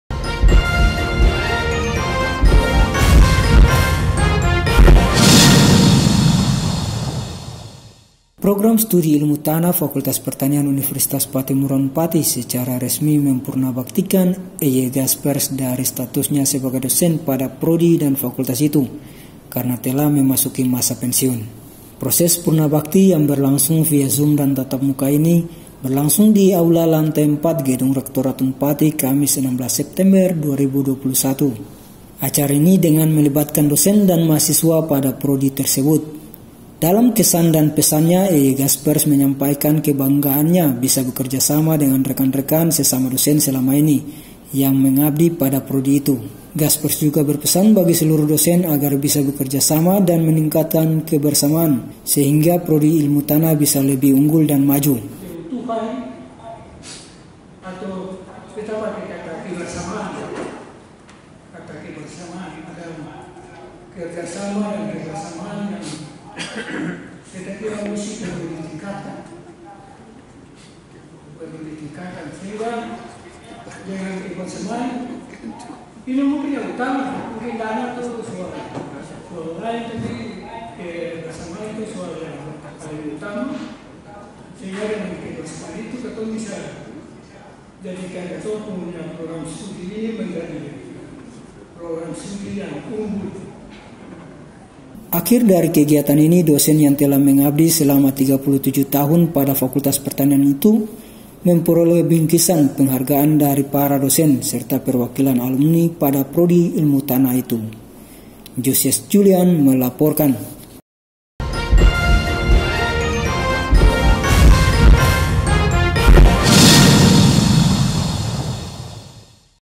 PATTIMURA TV - Prodi Ilmu Tanah Resmi Purnabaktikan Dosen GASPERSZ
Dapatkan Juga Informasi Terbaru Dan Terupdate Kami di :

Website kami